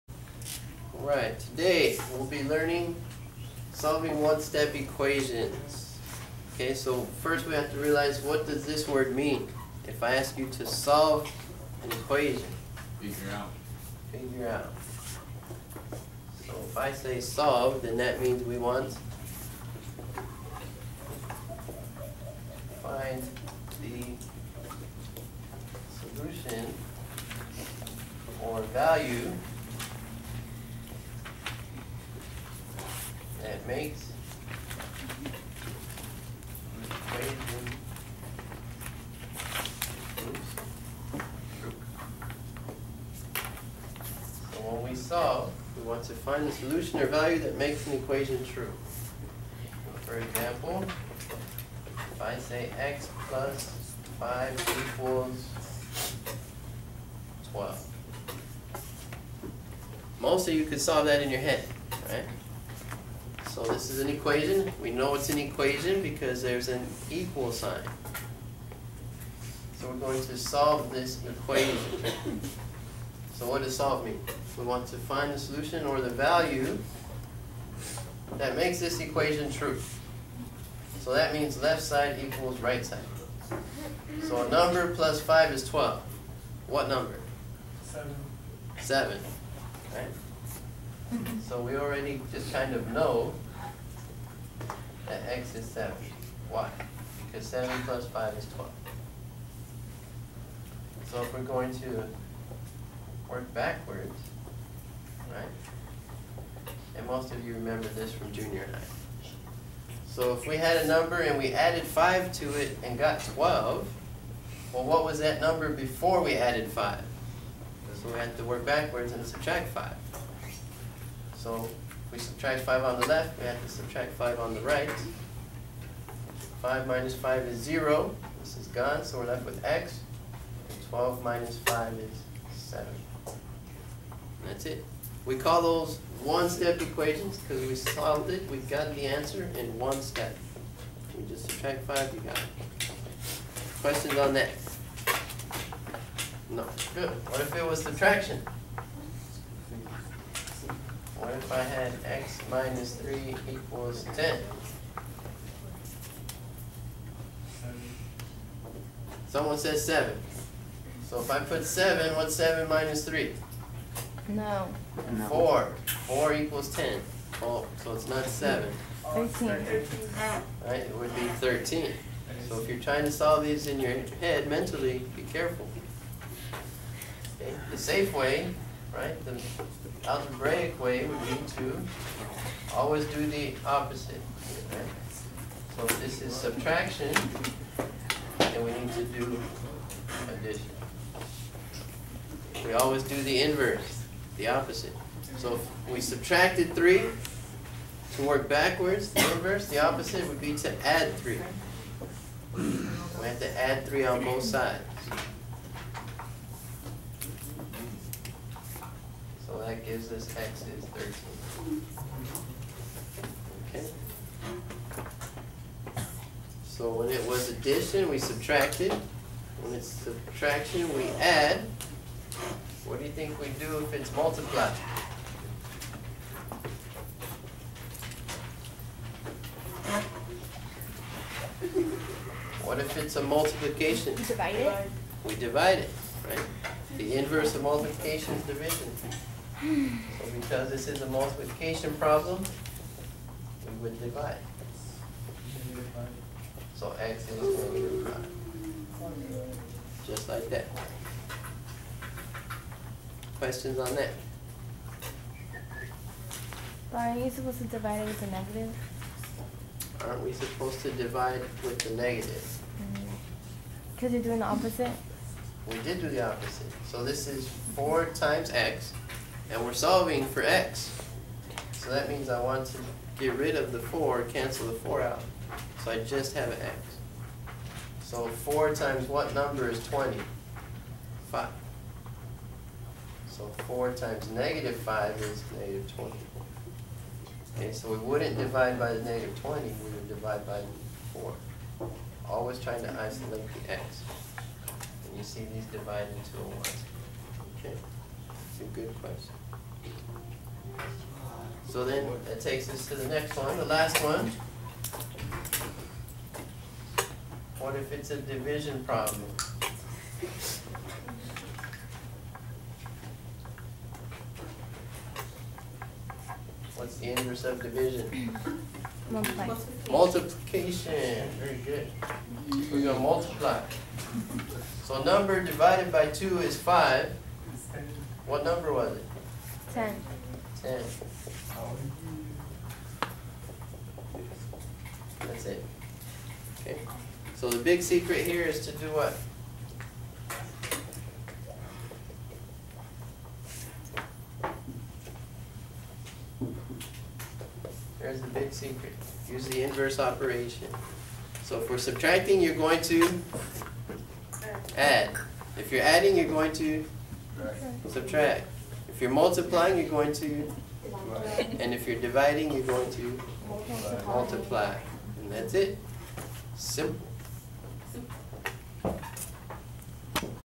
Solving 1-step Equations P1
Examples of how to solve 1-step equations.